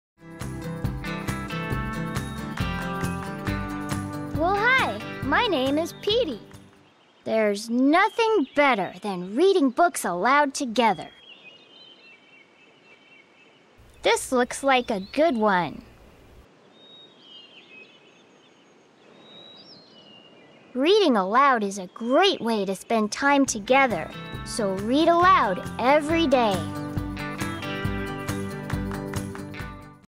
The Napping House - Letter N
P.T. suggests a book to read aloud.